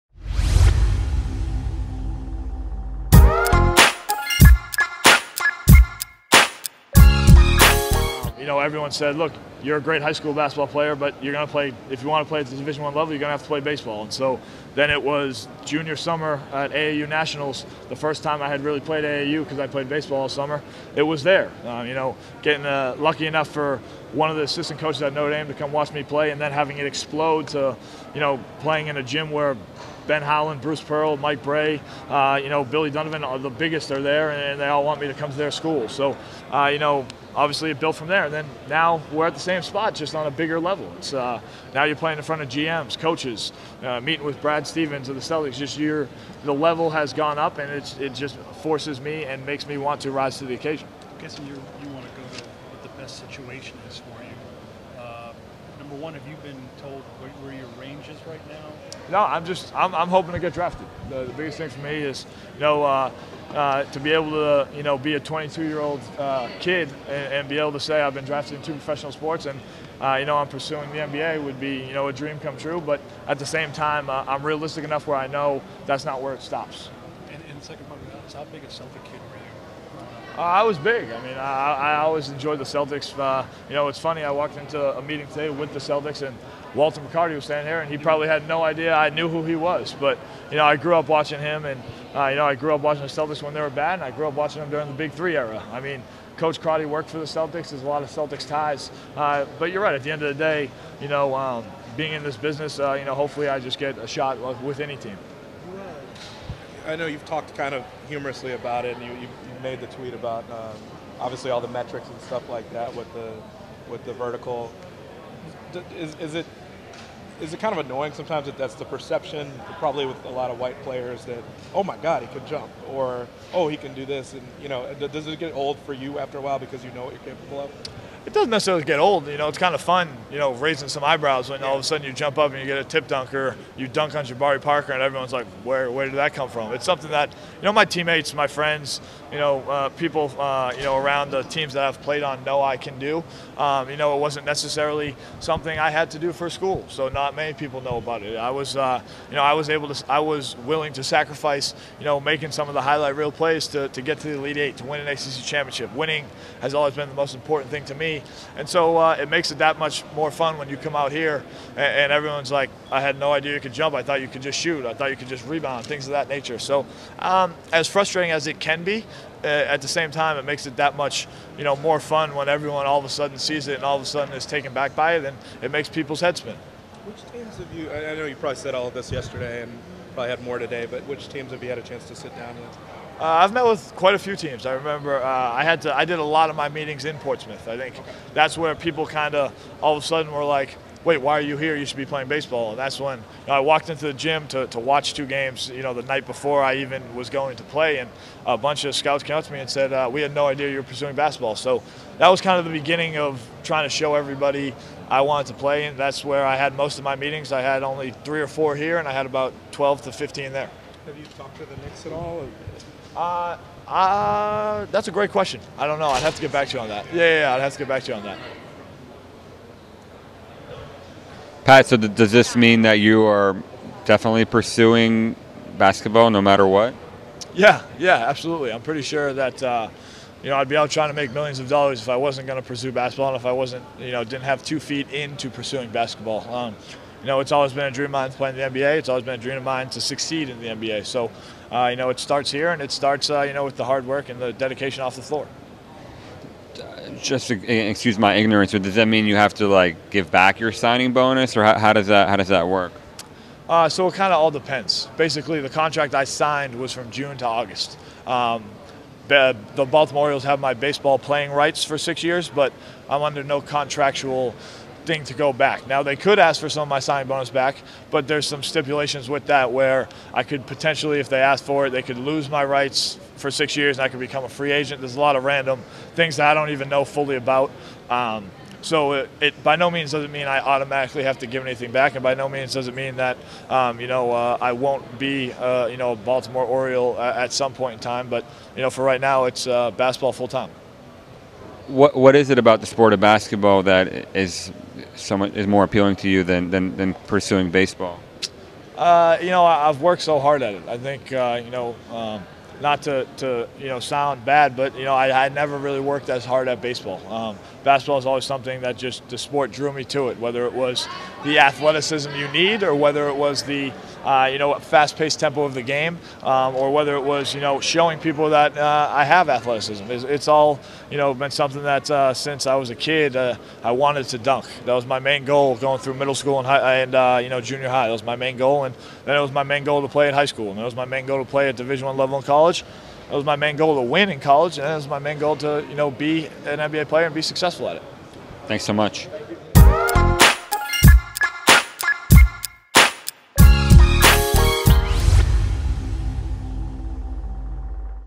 Pat Connaughton 2015 NBA Draft Combine Interview
Notre Dame guard Pat Connaughton discusses the 2015 NBA Draft at the NBA Combine.